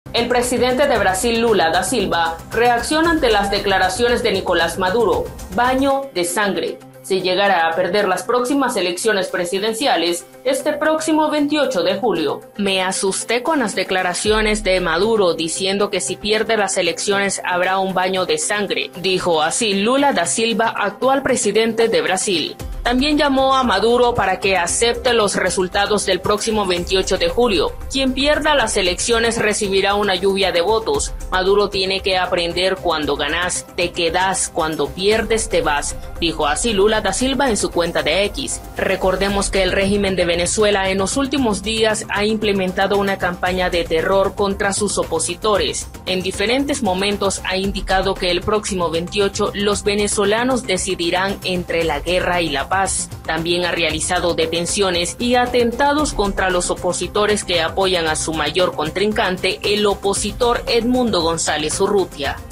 Lula Da Silva dice que Daniel Ortega no le responde el teléfono
El presidente brasileño, Luiz Inácio Lula da Silva, declaró que su homólogo nicaragüense, Daniel Ortega, no le responde las llamadas desde que el papa Francisco le pidió que intercediera por el obispo Rolando Álvarez, ahora en el exilio.